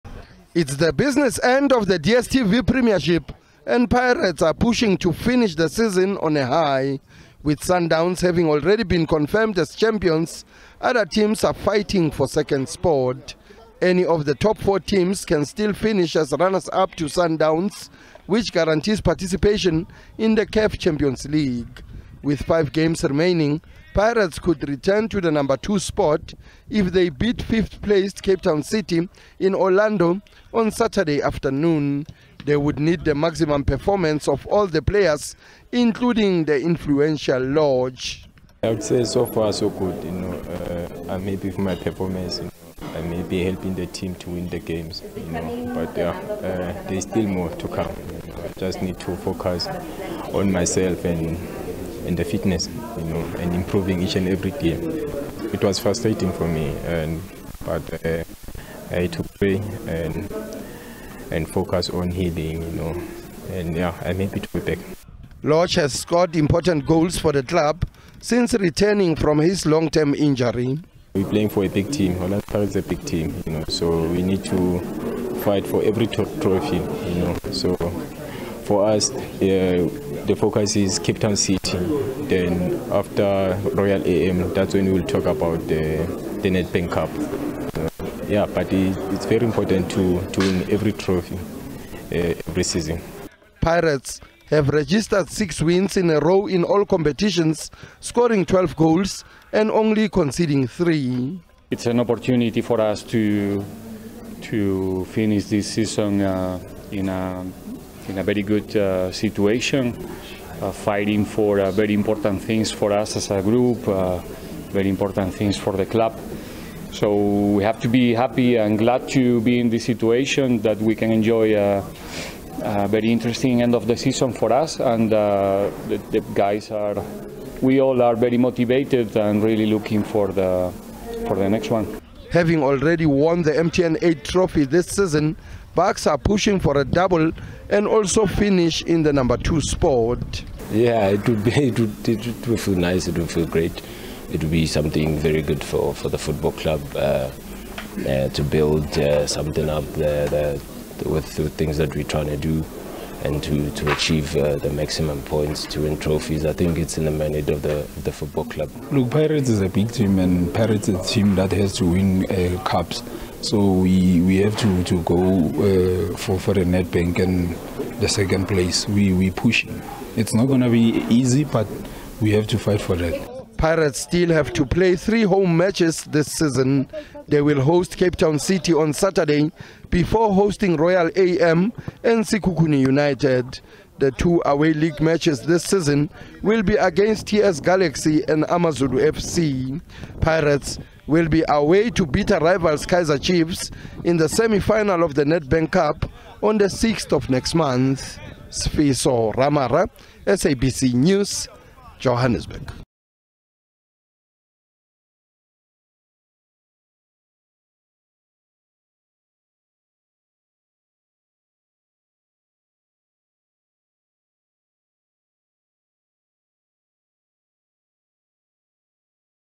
Pirates' Thembinkosi Lorch says the best is yet to come from him this season
Orlando Pirates attacker and talisman Thembinkosi Lorch says the best is yet to come from him this season. Lorch is back in full fitness after being sidelined by a long-term injury that ruled him out of action for five long months. The 29-year-old player is expected to play in an important role for his club with just a few games left before the end of the current season.